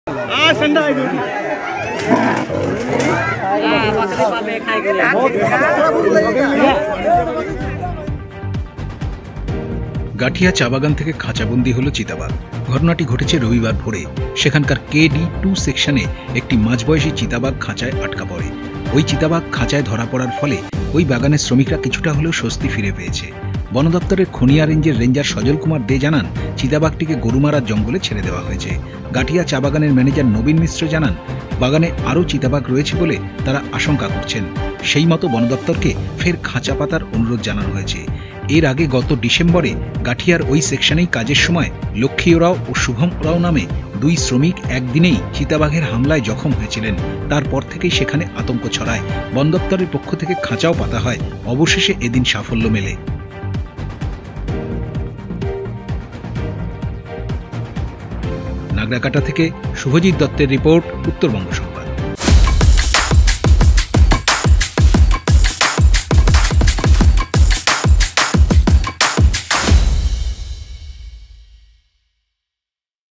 Lepoourd Captured | খাঁচাবন্দী চিতাবাঘ
গাঠিয়া চা বাগান থেকে খাঁচাবন্দি হল চিতাবাঘ। ঘটনাটি ঘটেছে রবিবার ভোরে। সেখানকার কেডি-টু সেকশনে একটি মাঝবয়সী চিতাবাঘ খাঁচায় আটকা পড়ে।